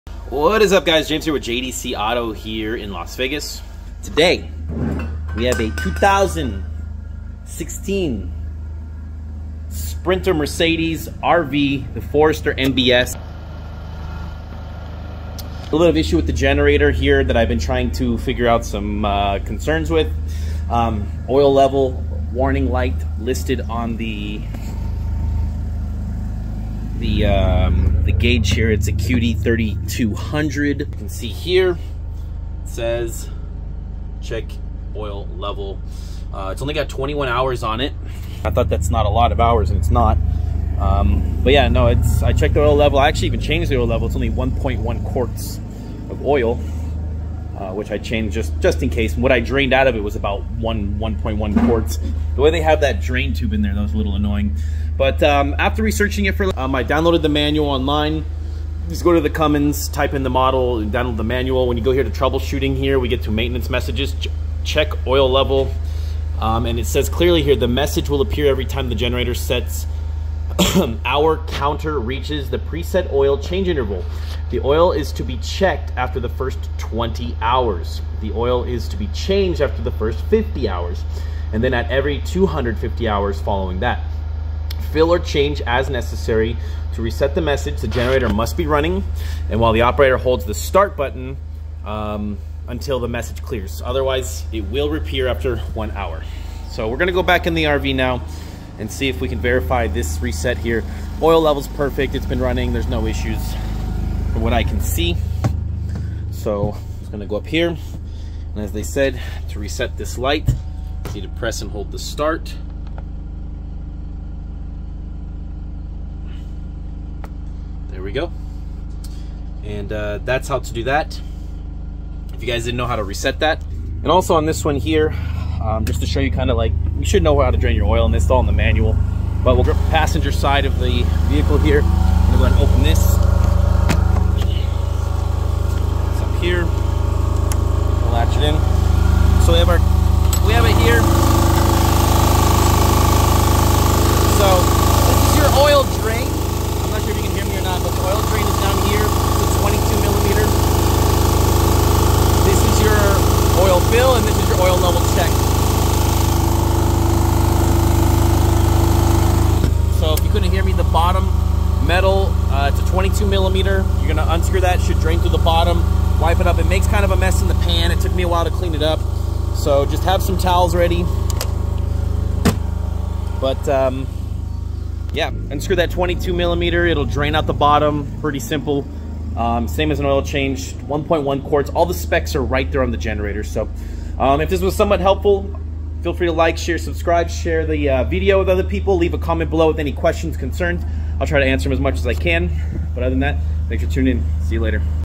Cummins QD 3200 RV Gen. CHECK OIL? Why and how to reset
automobile lasvegas mechanic 2016 Mercedes-Benz 3500 Sprinter Forester RV with a Cummins 3200 generator check engine on interface with only 20 hours how to reset the check oil light.